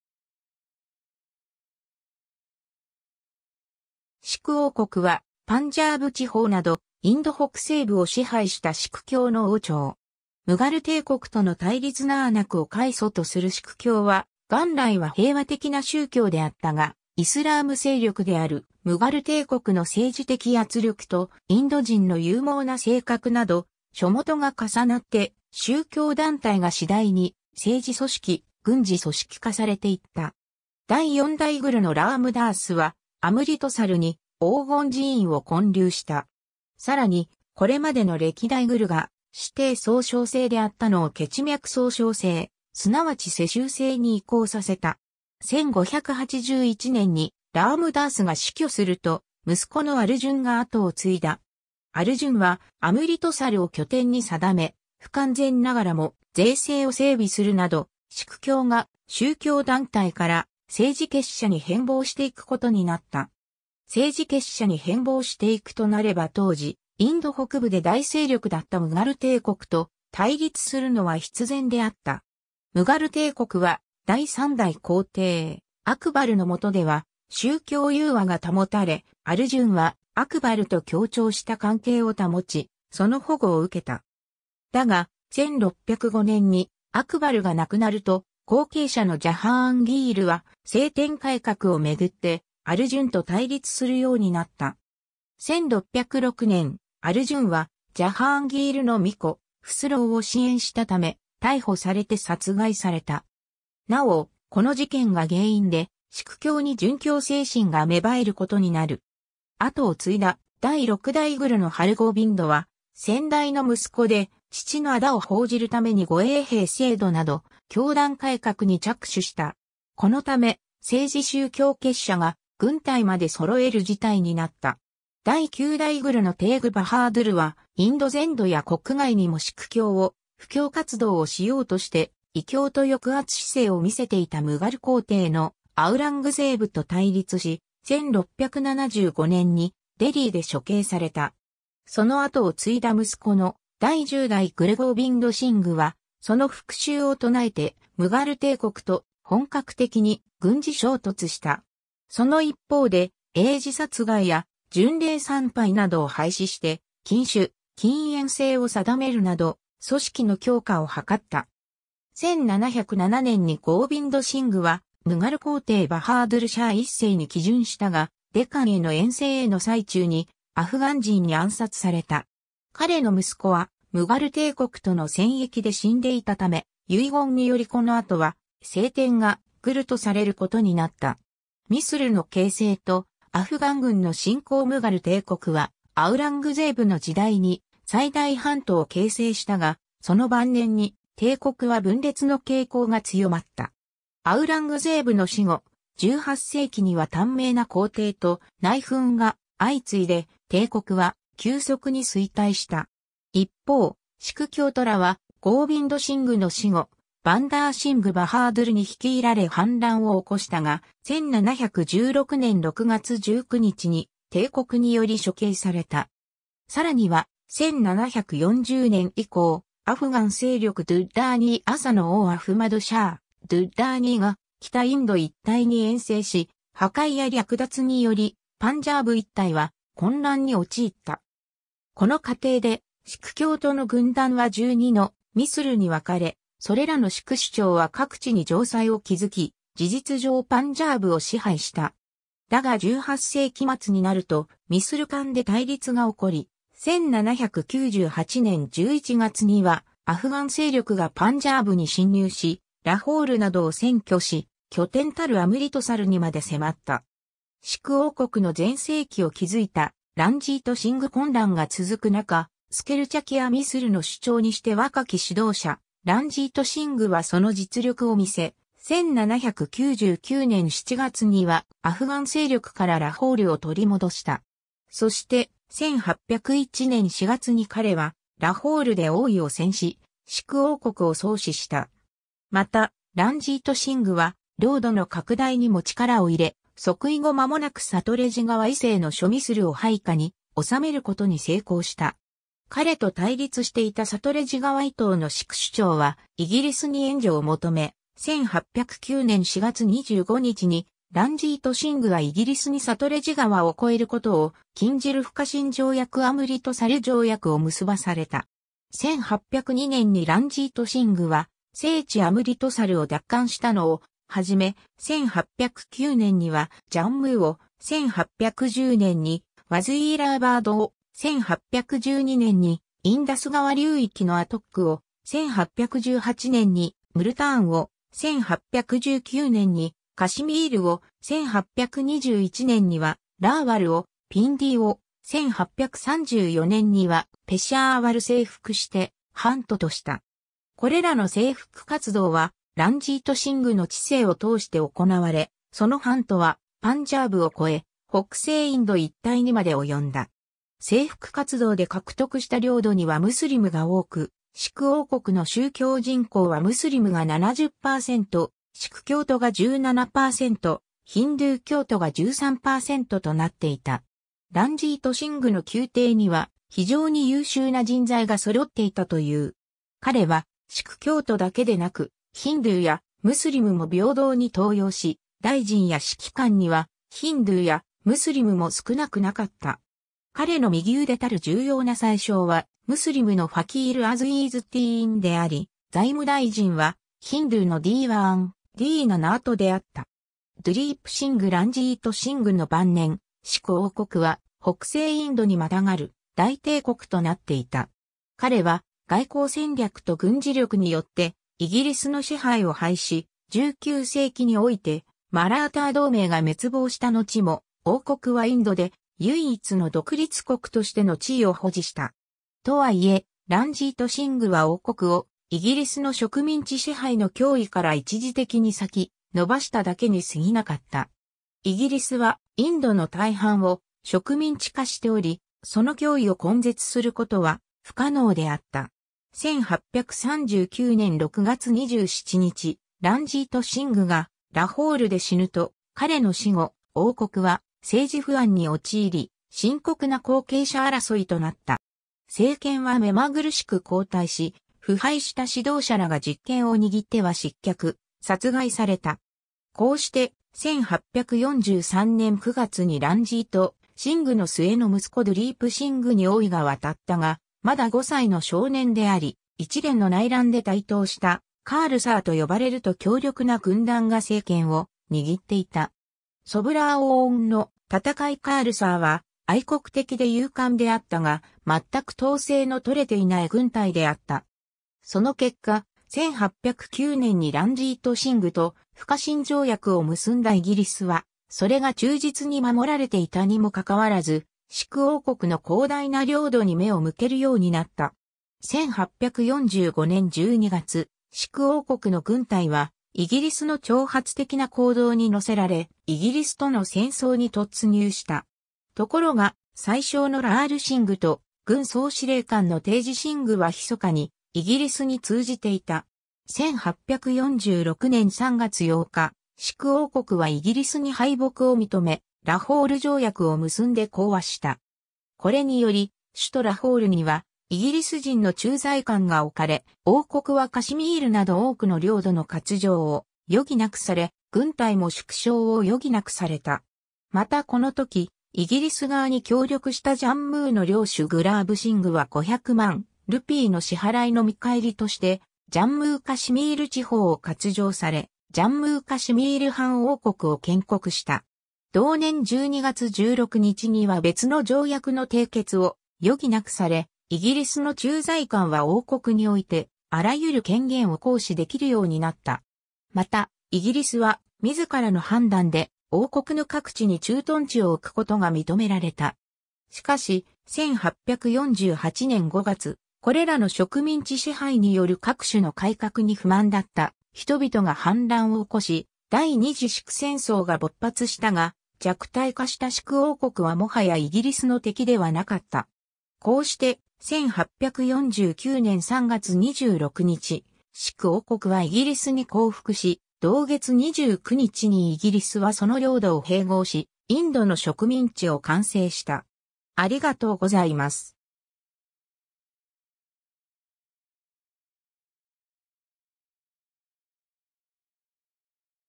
シク王国
シク王国（シクおうこく、1801年 - 1849年）は、パンジャーブ地方などインド北西部を支配したシク教の王朝。
 ムガル帝国との対立 ナーナクを開祖とするシク教は、元来は平和的な宗教であったが、イスラーム勢力である ムガル帝国の政治的圧力と、インド人の勇猛な性格など諸因が重なって宗教団体が次第に政治組織・軍事組織化されていった。
 第4代グルのラーム・ダースはアムリトサルに黄金寺院を建立した。
さらに、これまでの歴代グルが師弟相承制であったのを血脈相承制、すなわち世襲制に移行させた。
 1581年にラーム・ダースが死去すると、息子のアルジュンが跡を継いだ。
アルジュンはアムリトサルを拠点に定め、不完全ながらも税制を整備するなどシク教が宗教団体から政治結社に変貌していくことになった。
政治結社に変貌していくとなれば当時、インド北部で大勢力だったムガル帝国と対立するのは必然であった。
 ムガル帝国は第3代皇帝アクバルのもとでは宗教融和が保たれ、アルジュンはアクバルと協調した関係を保ち、その保護を受けた。
だが、1605年にアクバルが亡くなると、後継者のジャハーンギールは聖典改革をめぐってアルジュンと対立するようになった。
 1606年、アルジュンはジャハーンギールの皇子フスローを支援したため、逮捕されて殺害された。
なお、この事件が原因でシク教に殉教精神が芽生えることになる。
 跡を継いだ第6代グルのハルゴーヴィンドは先代の息子で、父の仇を報じるために護衛兵制度など教団改革に着手した。
このため、政治宗教結社が軍隊まで揃える事態になった。
 第9代グルのテーグ・バハードゥルはインド全土や国外にもシク教を布教活動をしようとして、異教徒抑圧姿勢を見せていたムガル皇帝のアウラングゼーブと対立し、1675年にデリーで処刑された。
 その跡を継いだ息子の第10代グル・ゴーヴィンド・シングはその復讐を唱えて、ムガル帝国と本格的に軍事衝突した。
その一方で嬰児殺害や巡礼参拝などを廃止して、禁酒・禁煙制を定めるなど組織の強化を図った。
 1707年にゴーヴィンド・シングはムガル皇帝バハードゥル・シャー1世に帰順したが、デカンへの遠征への最中にアフガン人に暗殺された。
彼の息子はムガル帝国との戦役で死んでいたため、遺言によりこの後は聖典がグルとされることになった。
 ミスルの形成とアフガン軍の侵攻 ムガル帝国はアウラングゼーブの時代に最大版図を形成したが、その晩年に帝国は分裂の傾向が強まった。
アウラングゼーブの死後、18世紀には短命な皇帝と内紛が相次いで、帝国は急速に衰退した。
 一方、シク教徒らはゴーヴィンド・シングの死後、バンダー・シング・バハードゥルに率いられ反乱を起こしたが、1716年6月19日に帝国により処刑された。
 さらには、1740年以降、アフガン勢力ドゥッラーニー朝の王アフマド・シャー・ドゥッラーニーが北インド一帯に遠征し、破壊や略奪によりパンジャーブ一帯は混乱に陥った。
 この過程でシク教徒の軍団は12のミスルに分かれ、それらのシク首長は各地に城塞を築き、事実上パンジャーブを支配した。
 だが18世紀末になると、ミスル間で対立が起こり、1798年11月にはアフガン勢力がパンジャーブに侵入し、ラホールなどを占拠し、拠点たるアムリトサルにまで迫った。
 シク王国の全盛期を築いたランジート・シング 混乱が続く中、スケルチャキア・ミスルの首長にして若き指導者ランジート・シングは その実力を見せ、1799年7月にはアフガン勢力からラホールを取り戻した。
そして、1801年4月に彼はラホールで王位を宣し、シク王国を創始した。
 また、ランジート・シングは領土の拡大にも力を入れ、即位後まもなくサトレジ川以西の諸ミスルを配下に治めることに成功した。
彼と対立していたサトレジ川以東のシク首長はイギリスに援助を求め、1809年4月25日にランジート・シングはイギリスにサトレジ川を越えることを禁じる不可侵条約アムリトサル条約 (1809年)を結ばされた。
 1802年にランジート・シングは聖地アムリトサルを奪還したのをはじめ、1809年にはジャンムーを、1810年にワズィーラーバードを、1812年にインダス川流域のアトックを、1818年にムルターンを、1819年にカシミールを、1821年にはラーワルをピンディーを、1834年にはペシャーワル征服して版図とした。
 これらの征服活動はランジート・シングの治世を通して行われ、その版図はパンジャーブを越え、北西インド一帯にまで及んだ。